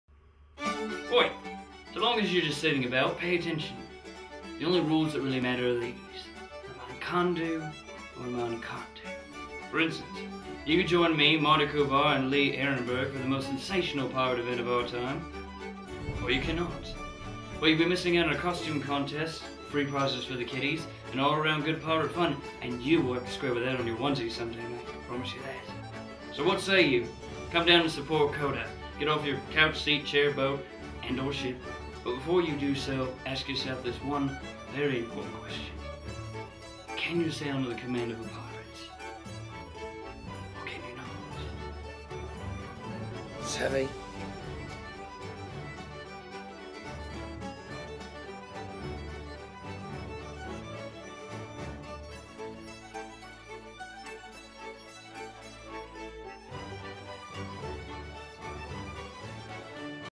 CoDA - Coalition for Dwarf Advocacy
The Coalition for Dwarf Advocacy (CoDA) was formed in late 2006 to:

*Advocate for the full rights and benefits of individuals with short stature (dwarfs), and their families within the public and private sectors of society;

*Focus its advocacy efforts to advance the rights and benefits of individuals with short stature (dwarfs), in the areas of medicine, vocation, education, adoption, and public accommodation; and

*Work with and assist other nonprofit organizations for the betterment of the lives of individuals with short stature (dwarfs) and to increase public awareness of the needs and abilities of such individuals.

Marty Klebba from the "Pirates of The Caribbean films, is one of the original founders of CoDA.

He along with co-star Lee Arenberg, will be having a Charity Event Aug 16 in Salt Lake City Utah. A Celebrity auction will also be held with items donated from such people as:

Orlando Bloom
Kiera Knightly
Naomie Harris
Kevin McNally

To see how you can help.

Every child deserves a home and a family...every child.